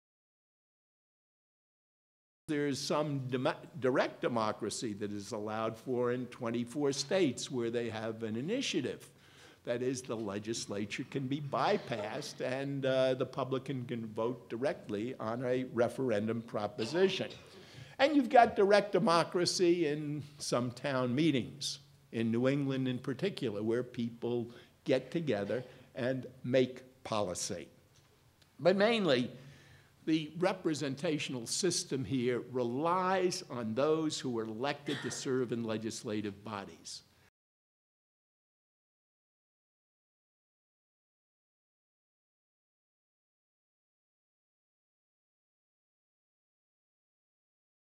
Alan Rosenthal Explains the Role of Direct Democracy in the United States (Discussion Topic 3)
Alan Rosenthal, Professor of Political Science, Eagleton Institute of Politics, Rutgers University, identifies examples of direct democracy in the United States. Viewers are asked to ponder whether the initiative process improves the representational process.

This video was part of a seminar devoted to the role of Congress during the July 2008 Alliance for Representative Democracy professional development initiative. The initiative explored the civics and government curricular programs of the Center for Civic Education, the Center on Congress at Indiana University, and the National Conference of State Legislatures.